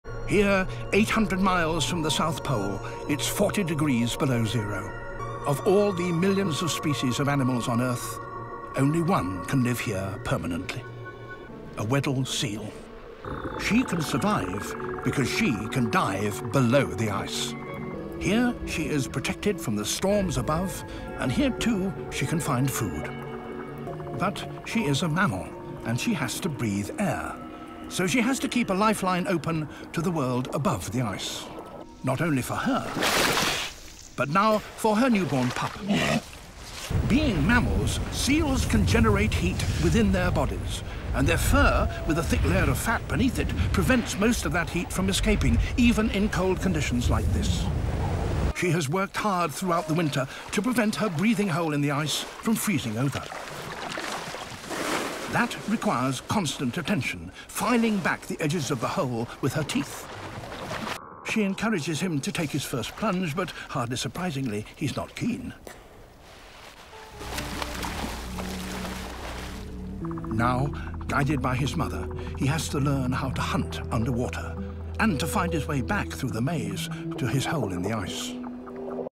How Weddell Seals Survive (800 miles from the south pole) Brutal Cold | Nature’s Toughest Survivor
How do Weddell seals survive one of the harshest places on Earth?

These incredible marine mammals thrive in Antarctica’s frozen wilderness, where temperatures drop below -40°C and the ocean is sealed under thick ice. In this video, discover the amazing adaptations that help the Weddell seal:
🔹 Hold its breath for over 80 minutes under ice
🔹 Dive up to 600 meters in freezing waters
🔹 Use its teeth to carve breathing holes in solid sea ice
🔹 Raise pups in extreme conditions

From their blubber insulation to their unique under-ice hunting skills, the Weddell seal is one of nature’s most resilient creatures.

📽️ Dive into the ice with us and witness how these seals defy the odds.